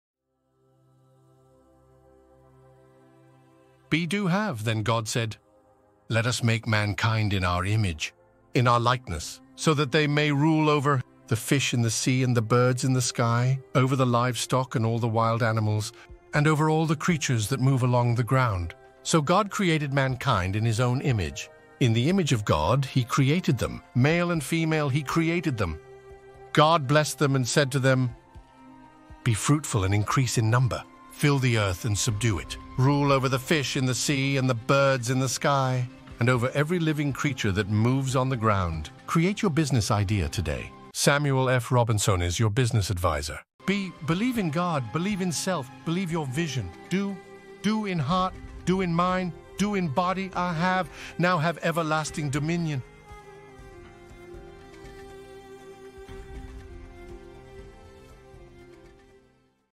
Unleash Your Power: How to Be, Do, and Have Dominion
In this powerful video, I'll help you unleash your power and take control of your life. I'll show you how to believe in God, self, and vision, and how to do work in a heart-mind-body way that will help you have dominion over your life.

If you're looking for ways to change your life, then this video is for you. I'll share with you how to access your power and take control of your life by believing in God,self, and vision. By doing work in a heart-mind-body way, you'll be able to have dominion over your life and live the life you're meant to live!

VidIQ helps you to expand your reach.